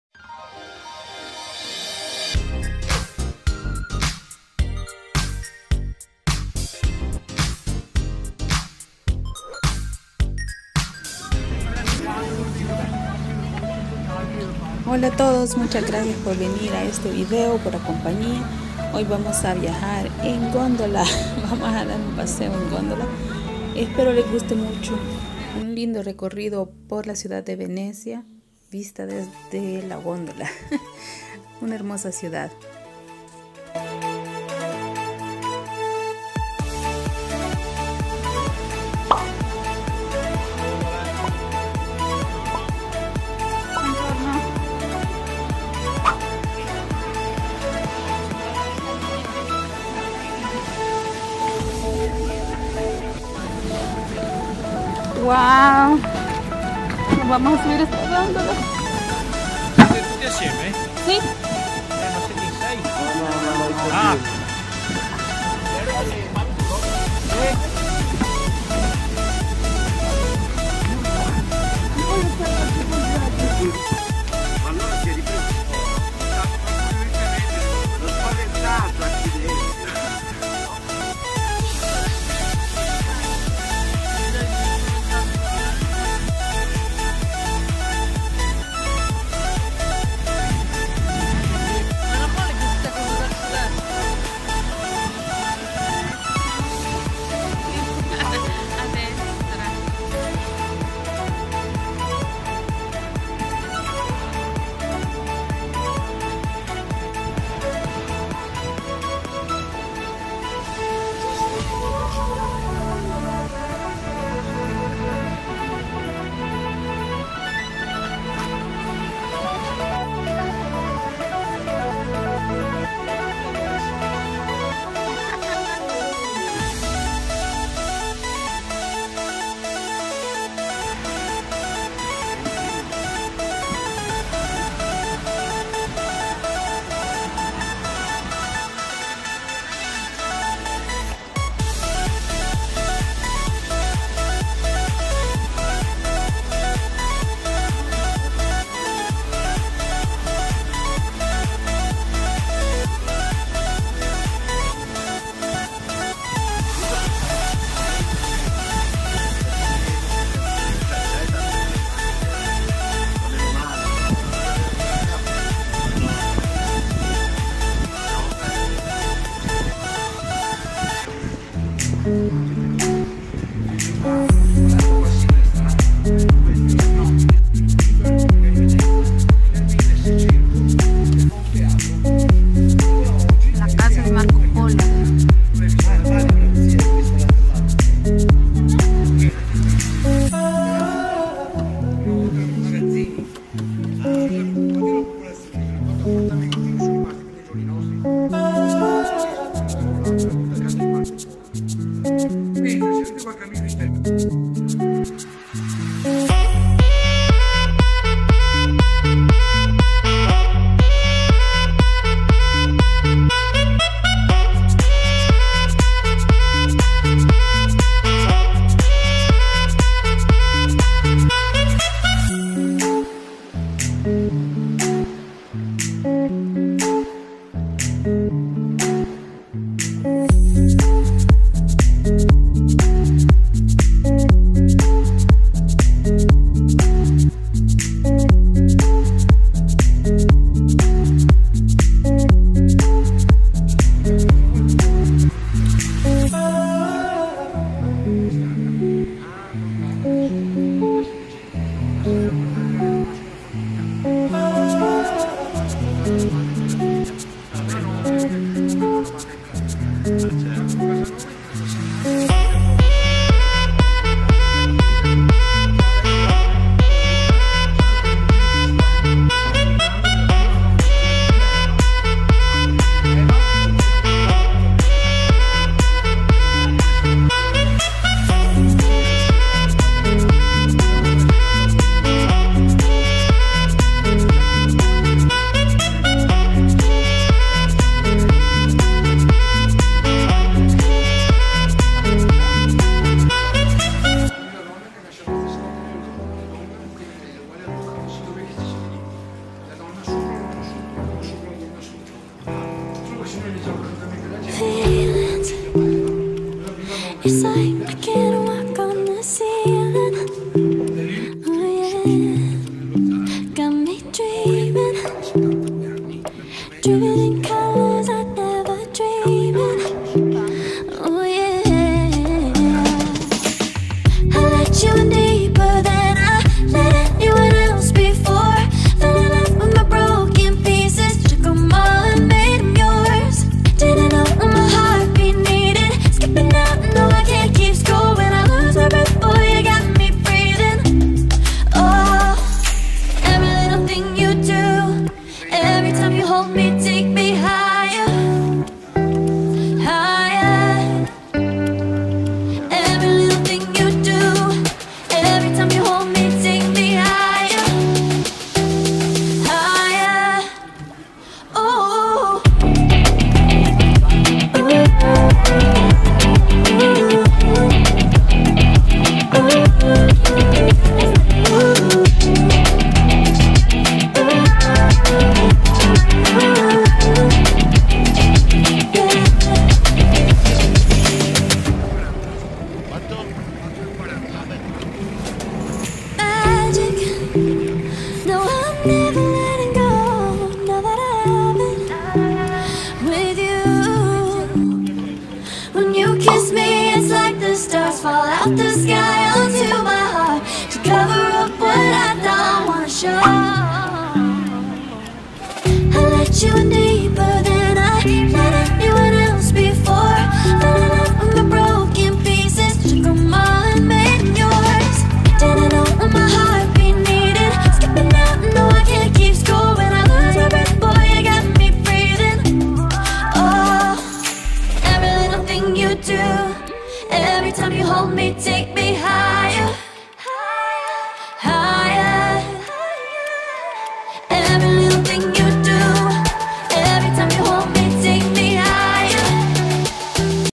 VENECIA EN GONDOLA, UN PASEO POR LOS CANALES
Un giro in Gondola per i canali di Venezia.

Daremis un paseo por lis canales de venecia en Gondola